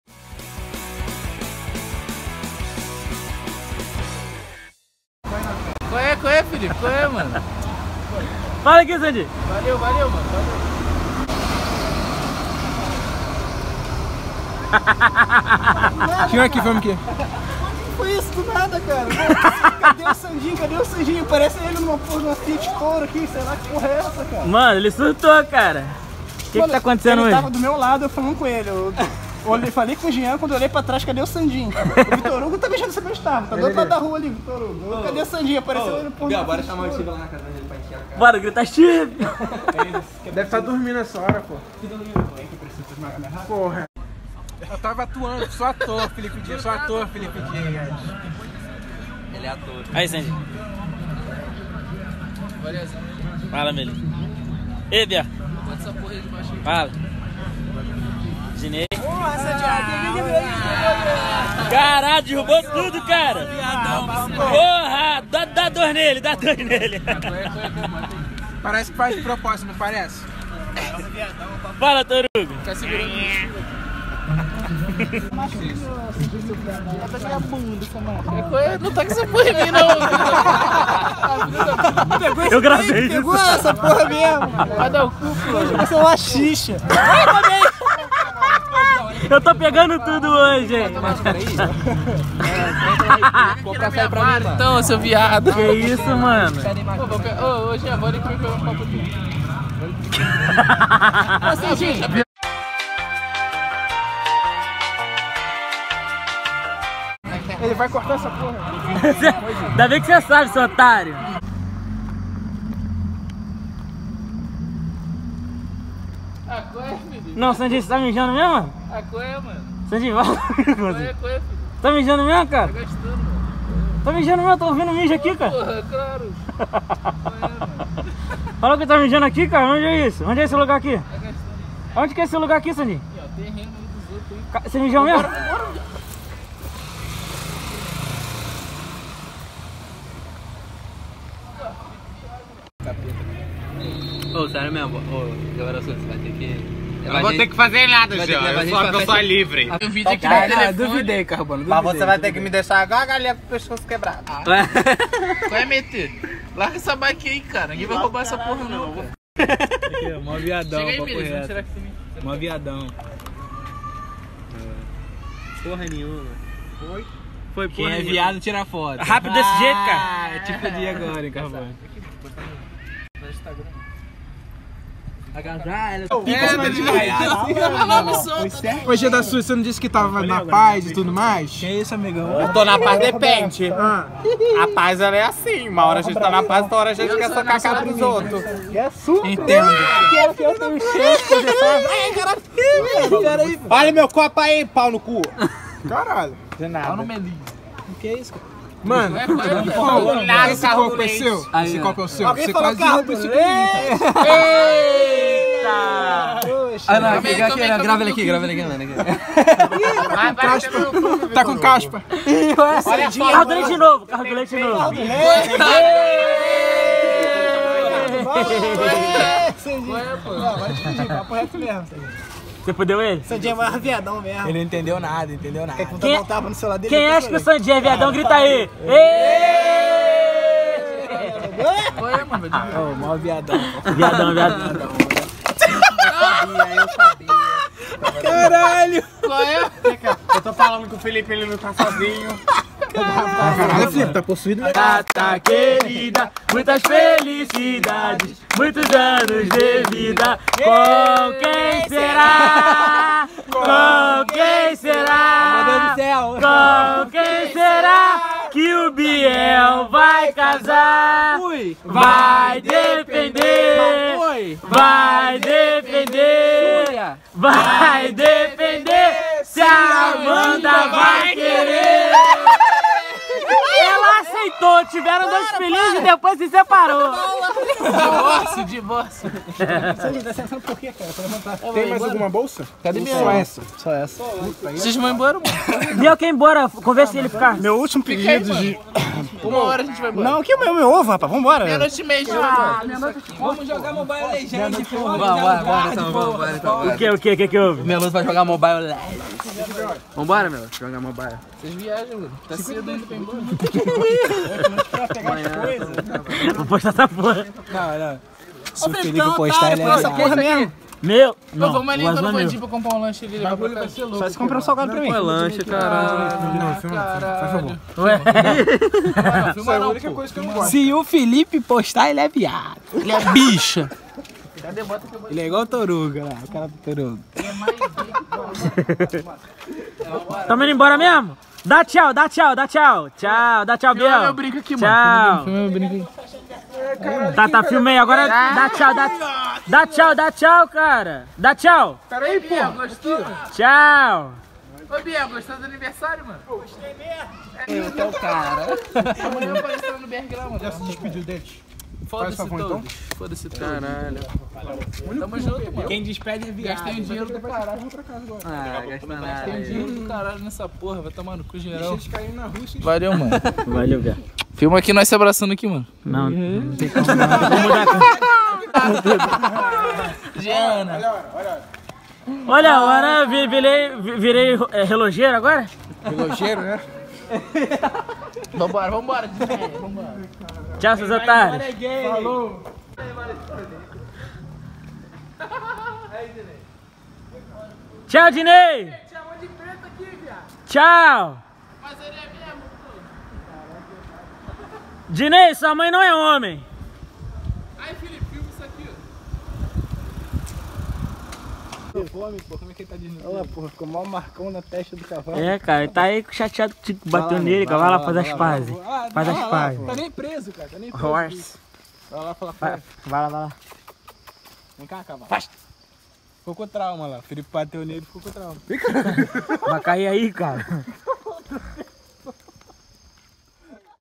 BAGUNÇA - resenha de aniversário do Biel (2021)
Quase como um evento anual, mais uma resenha de aniversário do Biel no ar! Muita merda, no meio da rua ainda por cima.